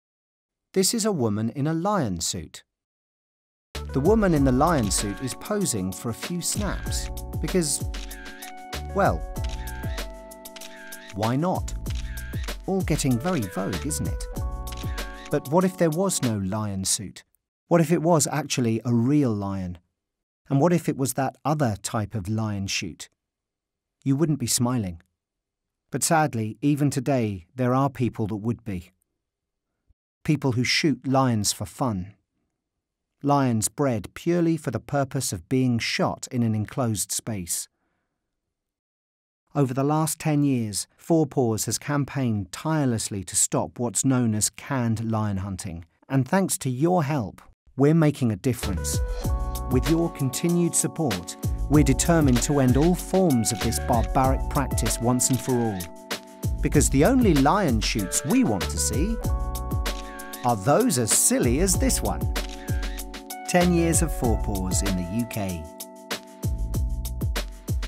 Lion Shooting – 10 Years of FOUR PAWS
You might think dressing up like a lion is a bit silly, but it’s all in the name of a very serious cause - FOUR PAWS is celebrating 10 years in the UK - and what a decade it’s been!

We opened our first ever Big Cat Sanctuary, LIONSROCK in South Africa, which is now home to over 100 rescued animals from around the world! We rescued the last remaining dancing bears in Europe, we even managed to shut down “the world’s worst zoo” in Gaza, rescuing all the remaining animals. Crucially, we have succeeded in helping thousands of animals in disaster zones all over the world.

So please share this with your friends on Facebook, Twitter or Instagram and help us to continue raising awareness of animal cruelty and help save thousands of animals across the world.